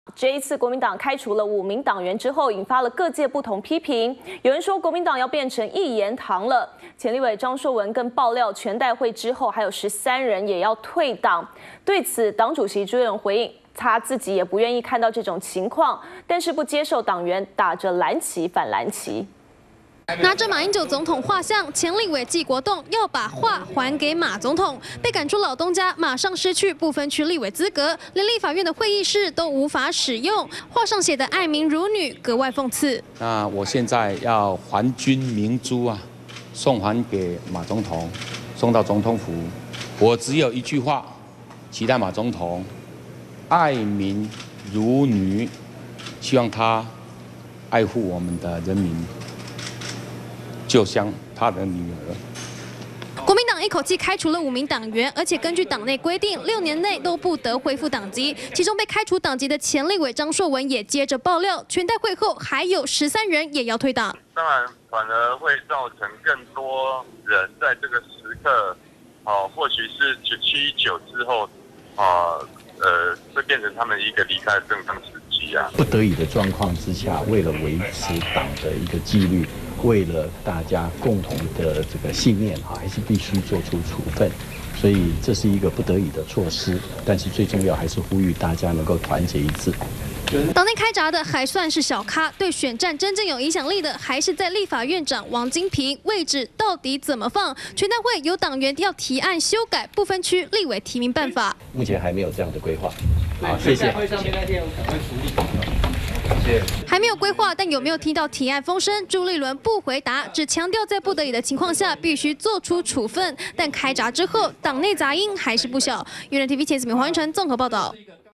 【2015.07.17】被開除黨籍 紀國棟把畫還給總統：還君明珠 -udn tv
國民黨開除五名黨員後，引發各界不同批評，有人說國民黨要變成一言堂，前立委張碩文更爆料全代會後還有13人也要退黨。對此黨主席朱立倫回應，自己也不願意看到這種情況，但不接受黨員打著藍旗反藍旗。完整內容 , FB , Twitter , G+